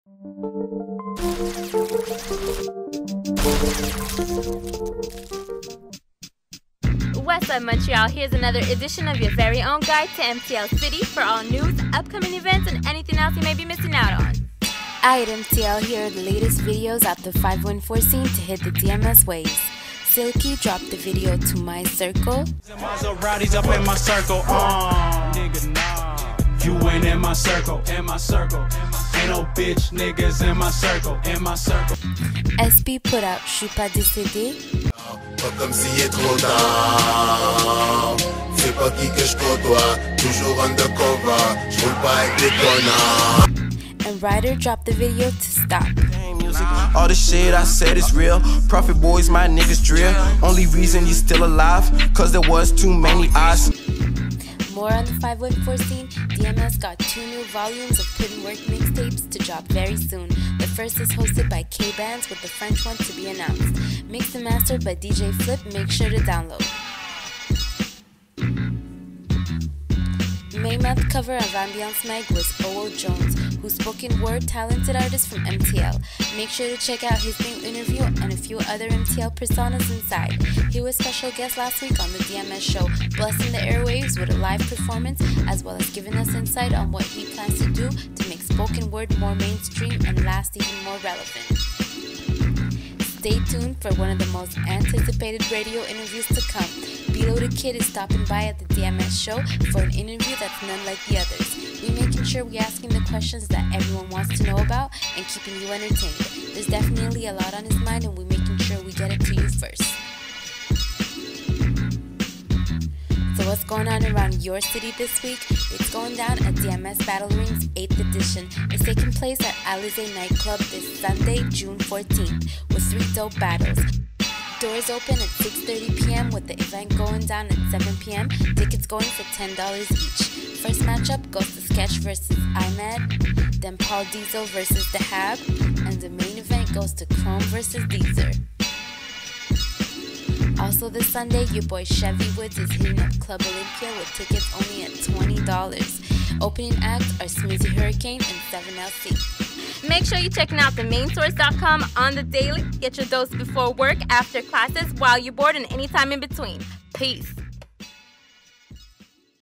DA MAIN SOURCE PRESENTS: DMS NEWS (WEEK 06/08/15)
(Talks About MTL New Rap Videos, Bilo Da Kid Interview, DMS Batte Ring June 14th event, Chevy Woods Show etc.. etc..)